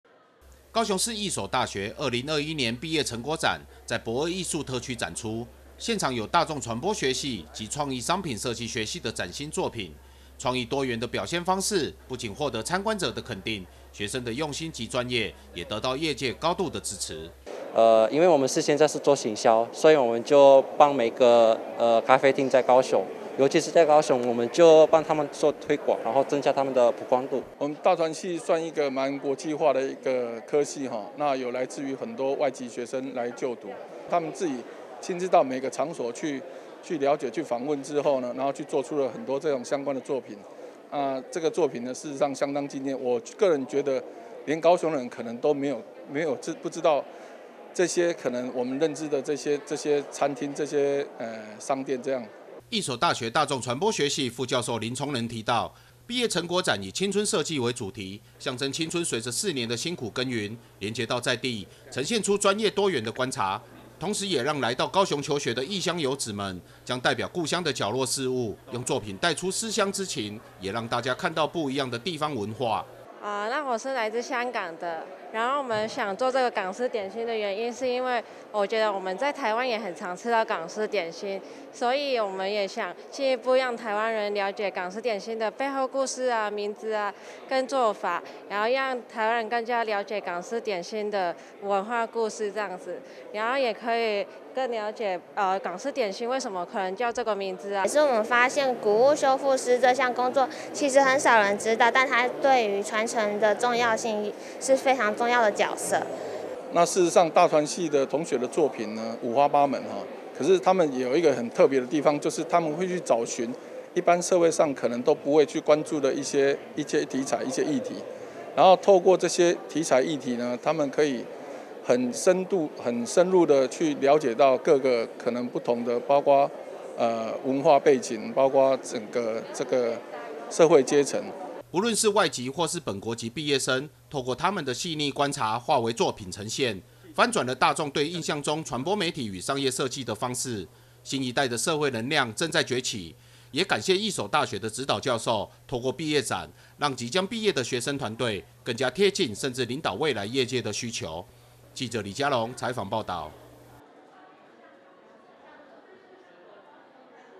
傳播設計新力量|傳播設計新力量 義守大學畢展創意驚艷
傳播設計新力量 義守大學畢展創意驚艷
2021年義守大學畢業展在駁二七號倉庫登場，傳播科系與商業計記系的畢業生們，用他們四年的時間觀察地方人文，甚至是民眾沒去注意到的角落化為作品呈現，每項完成品都令人驚艷，透過鏡頭讓我們去看看。